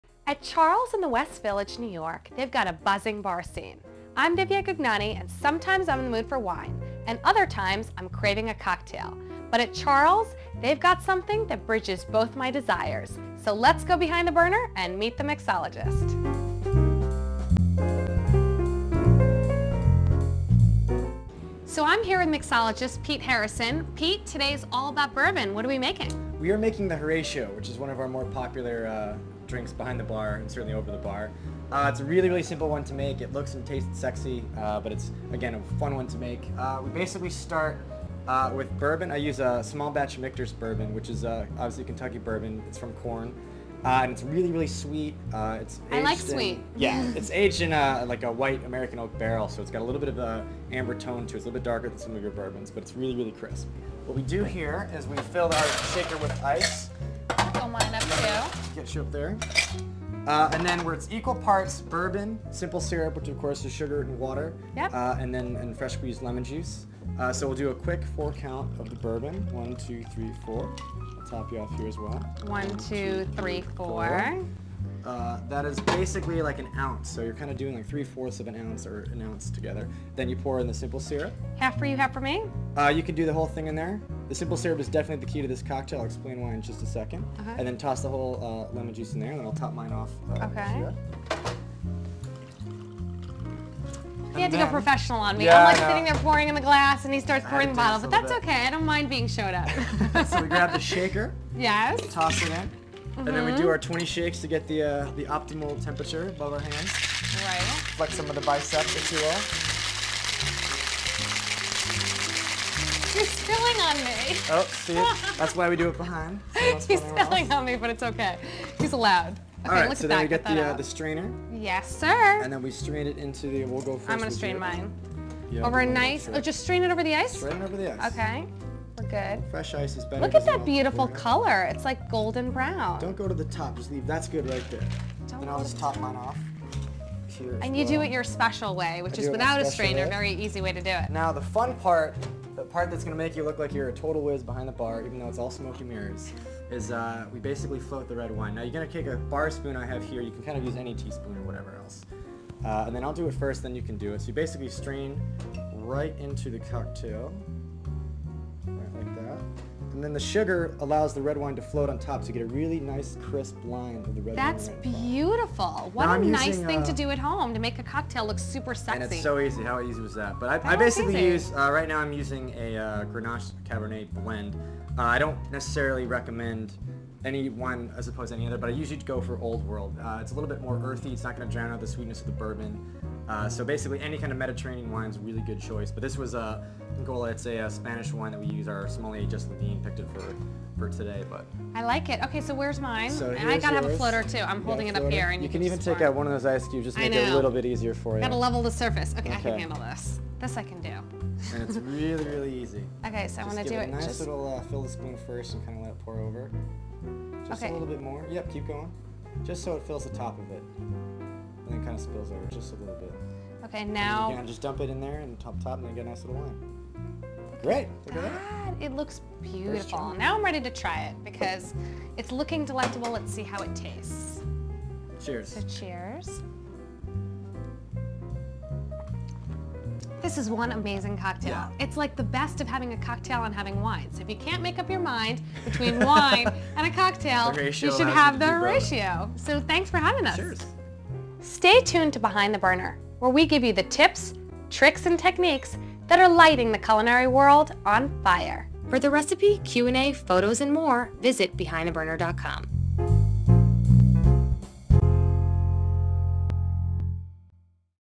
"The Horatio" Cocktail with Pete Harrison at Restaurant Charles
Master mixologist Pete Harrison at Restaurant Charles in Manhattan's West Village shakes up a refreshing neighborhood-inspired cocktail. If you're looking for a tasty cocktail before your dinner date, visit Pete and sample The Horatio, with a bold bourbon and sexy red wine floater.